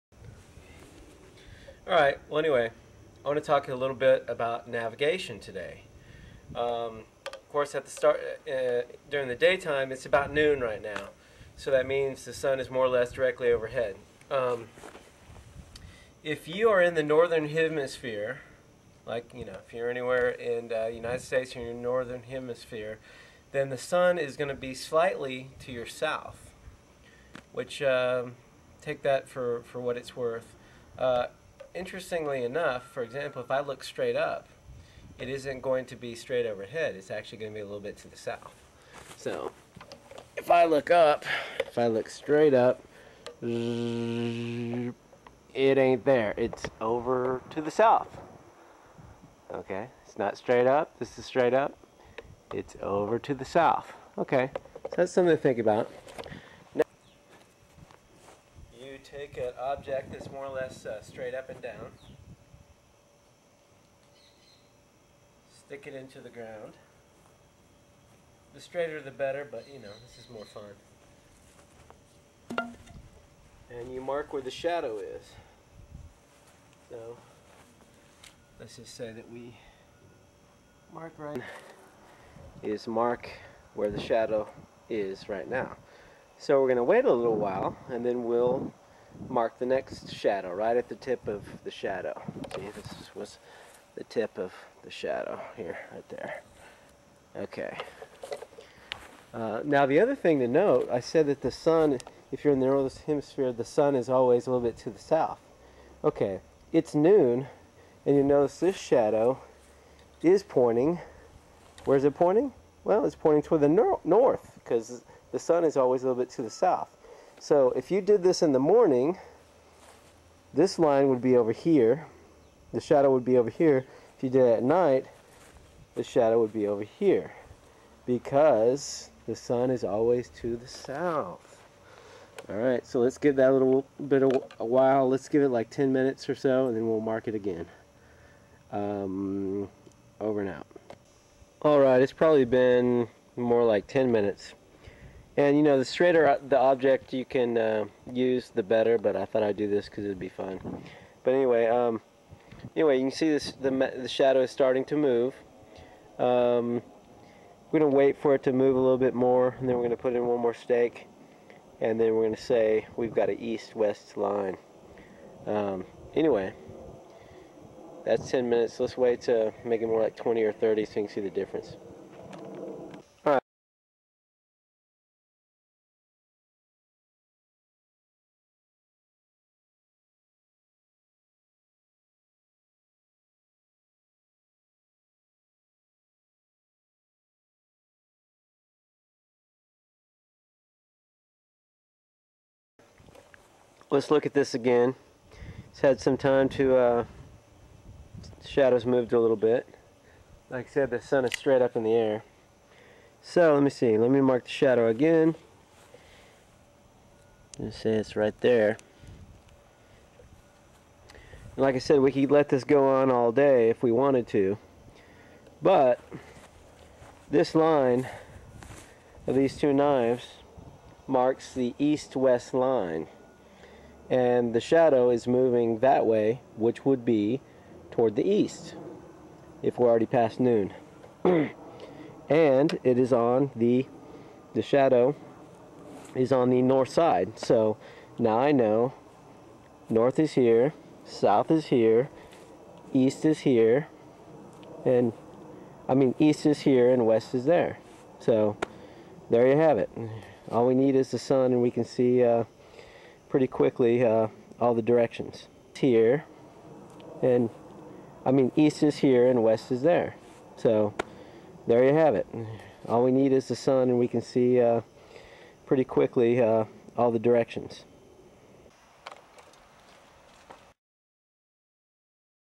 Navigation using the Sun even at noon
you can figure out north south east west all from the sun, even at noon when the sun is directly overhead